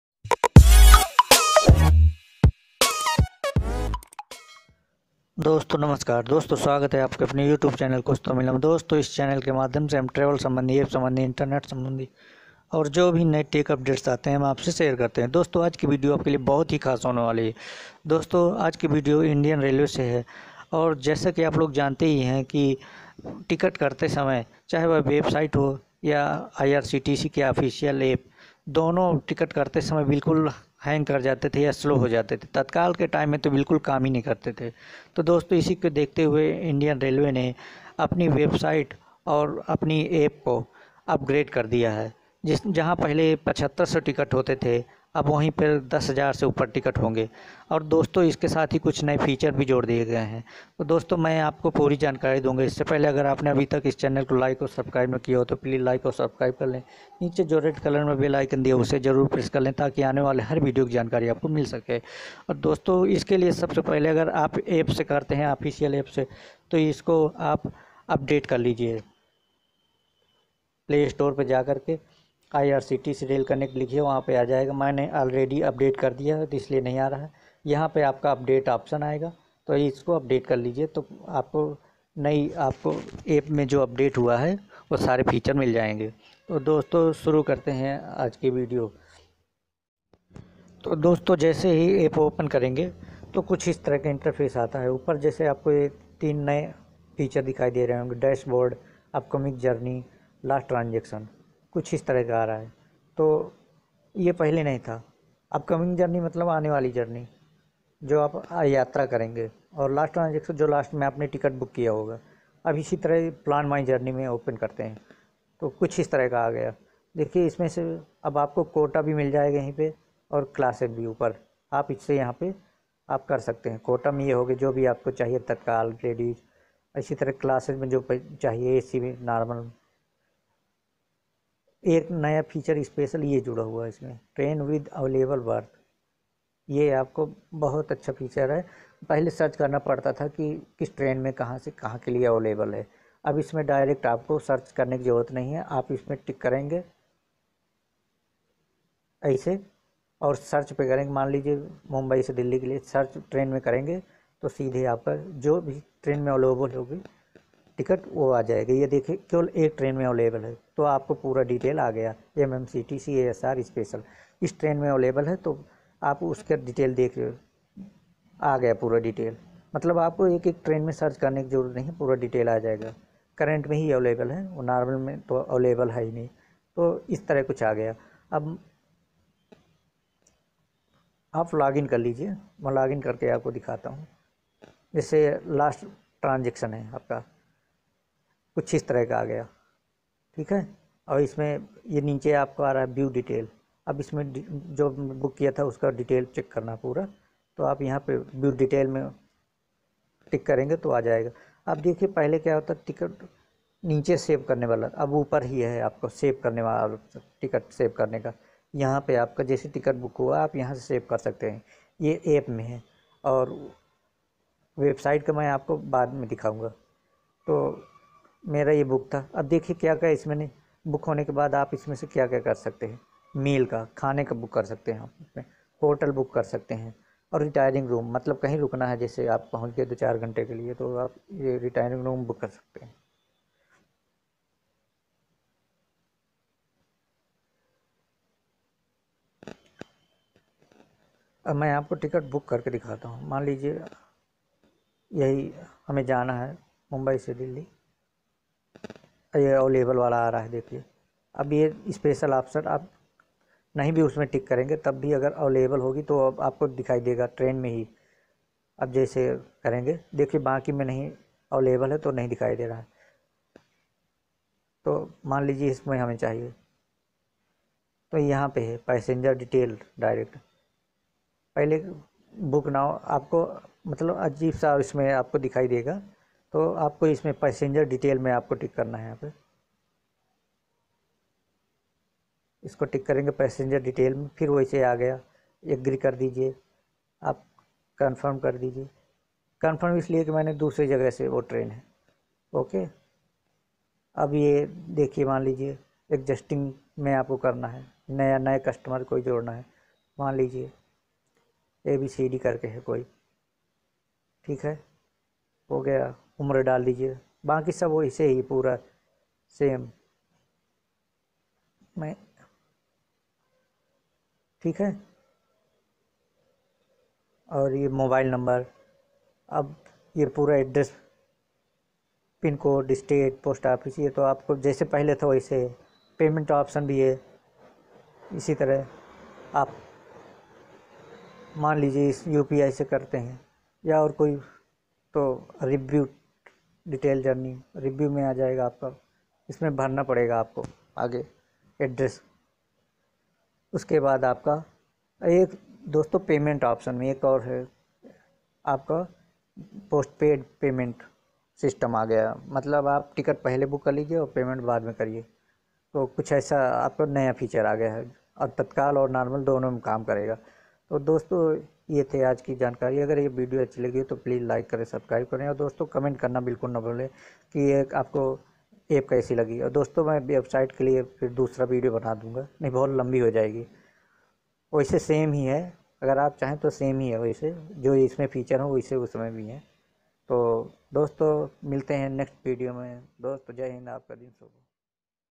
IRCTC Website & App Upgrade। IRCTC टिकट आसानी से बुक होंगे। IRCTC New Website 2021। Kuch To Mila.
IRCTC New Website Launched। Booking Tickets To Become Easy। IRCTC New Website 2021। Kuch To Mila.

      Dosto IRCTC  ne ab new year me new website aur IRCTC official App ko upgrade kar diya hai. is upgrade se tickets bahut hi easy book honge. Jaha pahle 7500 tickets book karne ki kshamta thi ab 10000 tickets booked honge.
        Iske sath hi kuch new features bhi add kar diye gye hai. jaise Meal, Hotel & Retiring room ki booking ab easily ho sakegi.

IRCTCNewWebsite2021 irctcupgradeWebsite&App KuchtoMila.